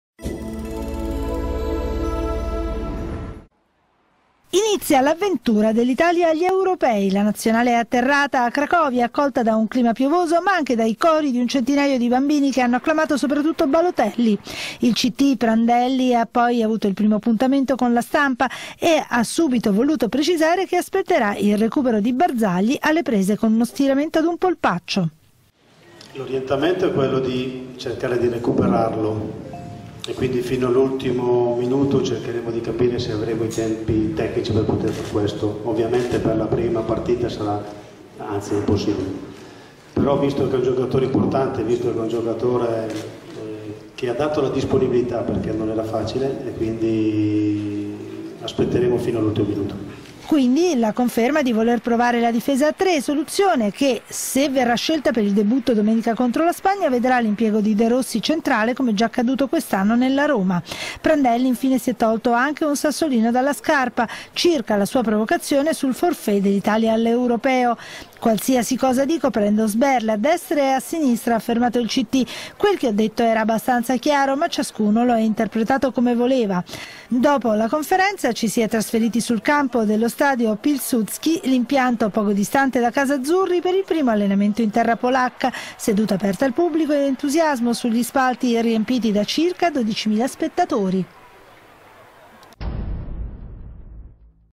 Italia - spagna HD 2012 Anteprima - Azzurri ben accolti in Polonia
Anteprima Italia - Spagna: formazioni e interviste. De rossi giocherà probabilmente centrale difensivo